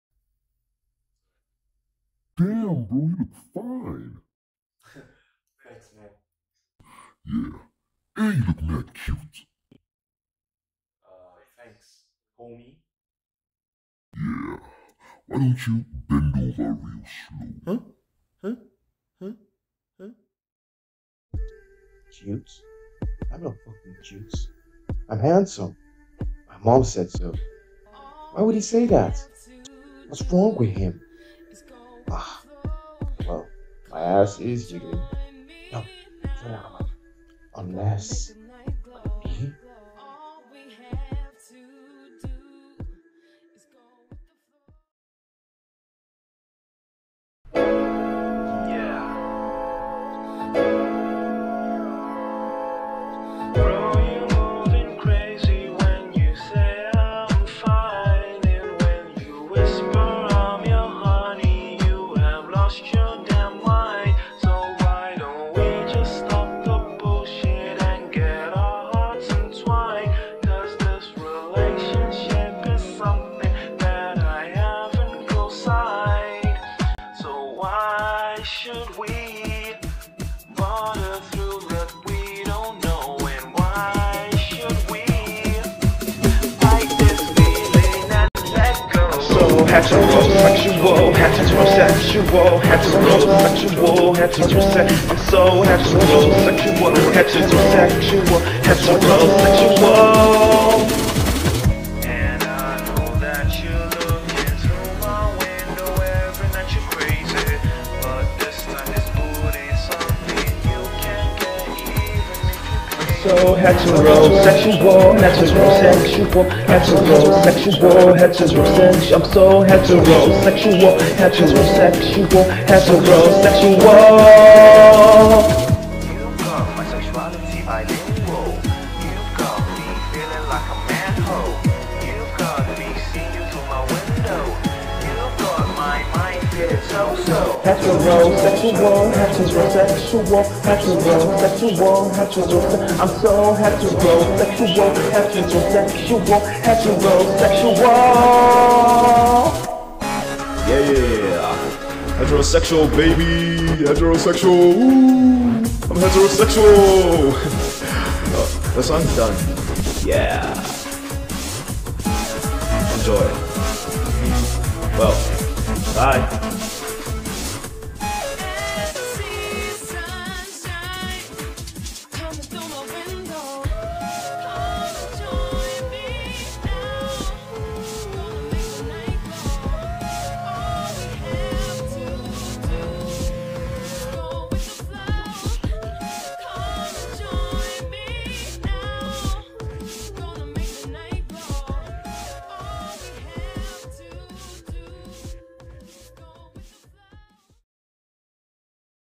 Heterosexual Music Video
Hi again, do people still read these? Anyway, always wanted to make a music video from scratch with the song, beats, edit, and everything else so I was like why not, enjoy.